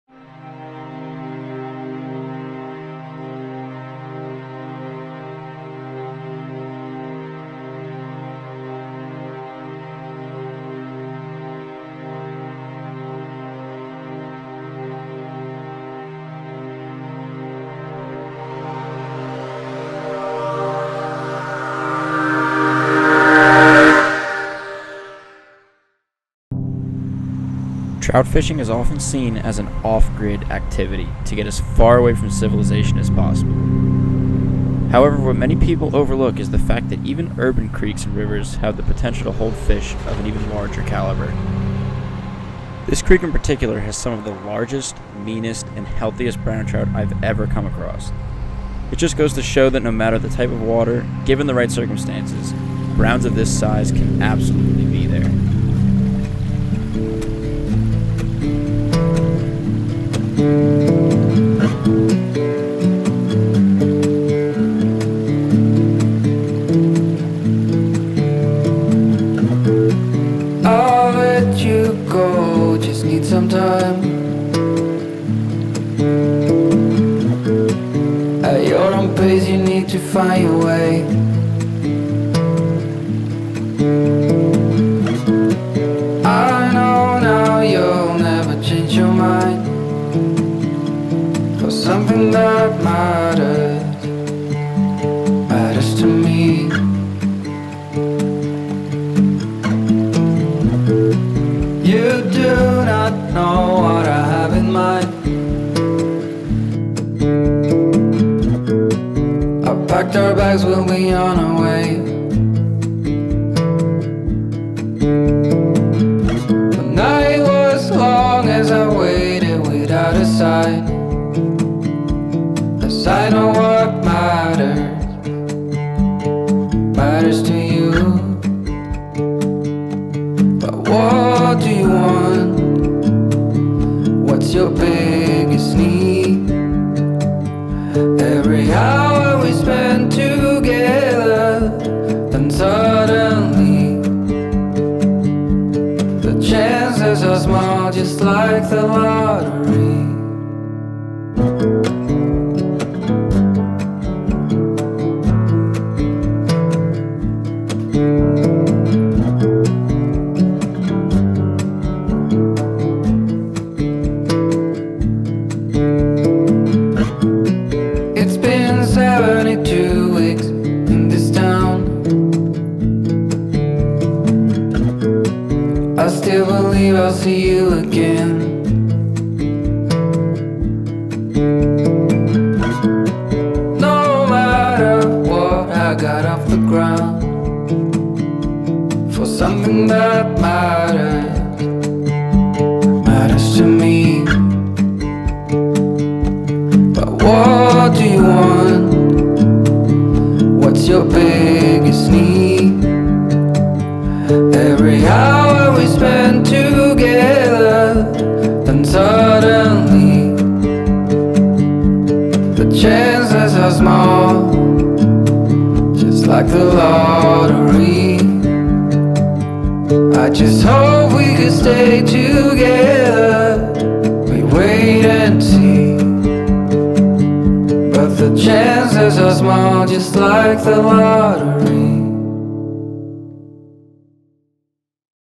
CITY LIMITS - Urban Fly Fishing for Wild Brown Trout
Wild trout aren't always off-grid and in the backcountry. Sometimes even in urban areas like these can hold massive fish! Get out there and find new waters!